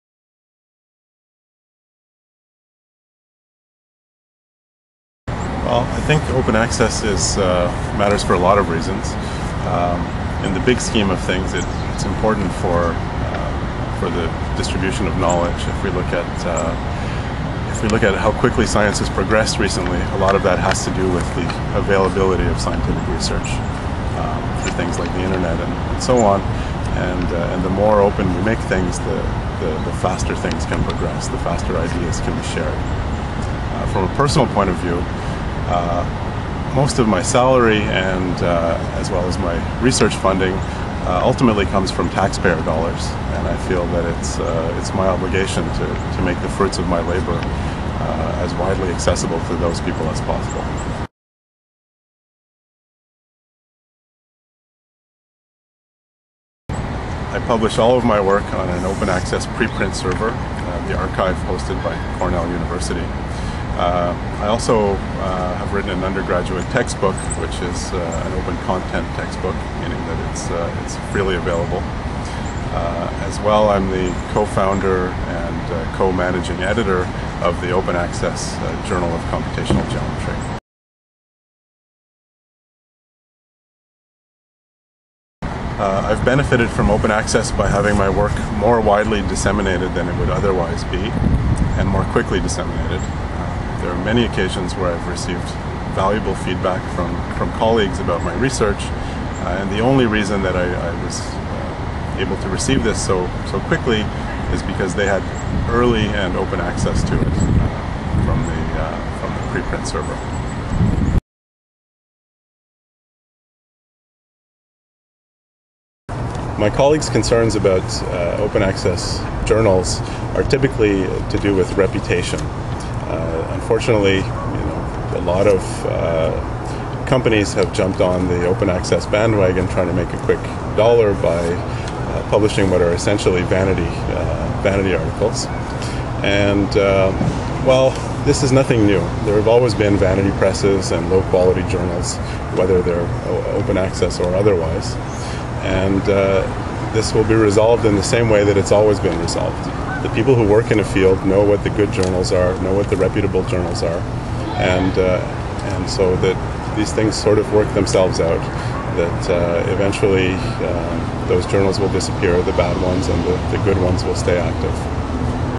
Pat Morin on Open Access
Dr. Pat Morin, Professor of Computer Sciences at Carleton University, discusses Open Access' advantages.
Thank you to the Carleton University library.